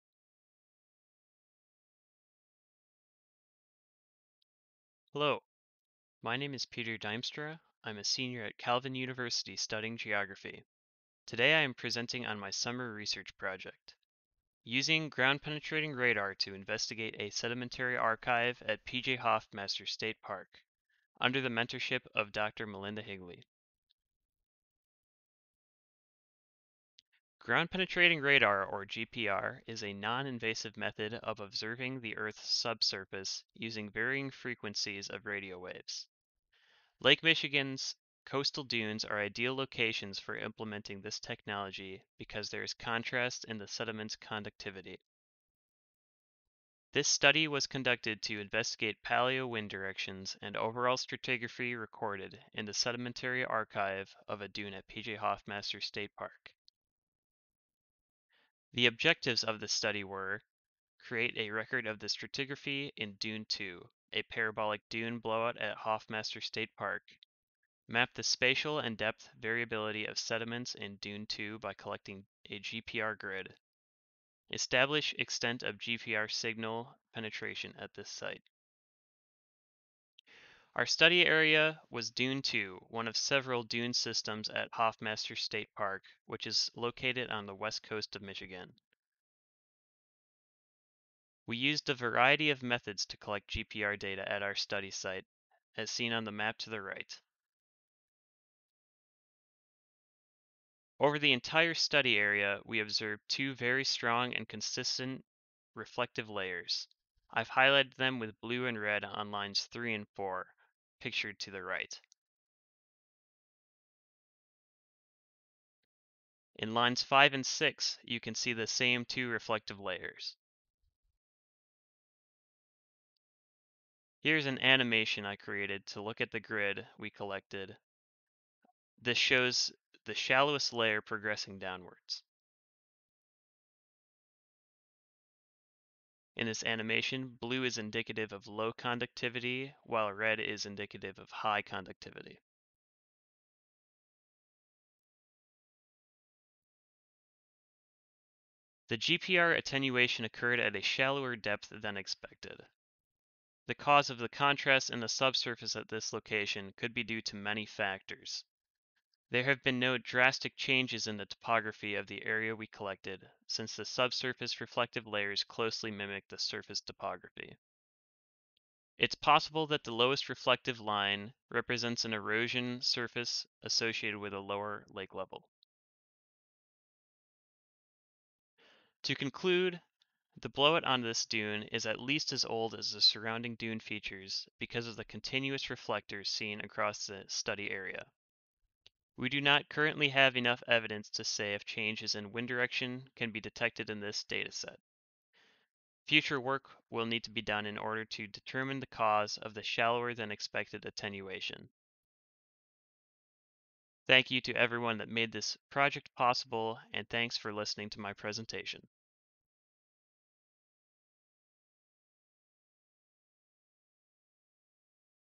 Using Ground Penetrating Radar to Investigate a Sedimentary Archive at PJ Hoffmaster State Park
By Peter Duimstra

Sedimentary layers can be deposited by waves or wind in a coastal dune system. These layers can be examined to track deposition and erosion in the dune’s history. Lake Michigan has a cycle of rising and falling lake levels, which means there will be changes recorded in its coastal dune structures. Understanding coastal structures is important because dunes are a vital asset to ecosystems and human developments on the coastline represent significant investments. Ground Penetrating Radar (GPR) provides a noninvasive method to examine the sedimentary layers below the surface. With this technology it is possible to model the dune subsurface which will be used to identify erosional and depositional events. This data will contribute to understanding how lake levels impact the formation and destruction of coastal land forms. Understanding these changes will assist management in preparation for the consequences that come with the volatile lake levels associated with climate change.